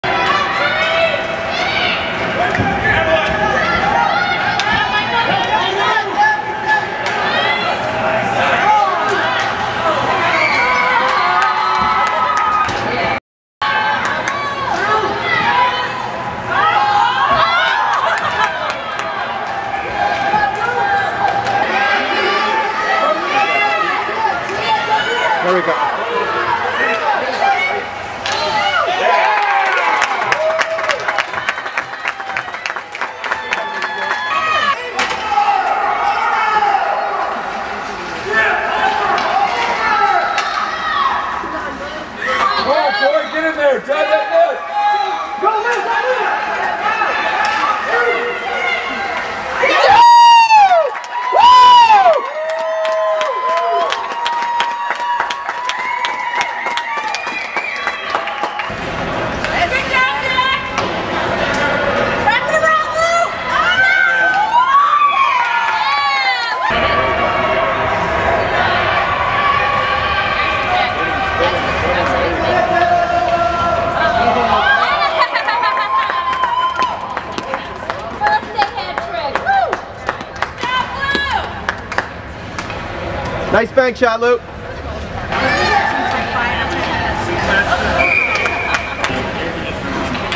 Messmer Nets Seven In Chicago Tourny
Lou Messmer (#67) celebrates his 9th birthday with teammates at the President's Cup Tournament in Chicago.  Included here are highlights from the four games.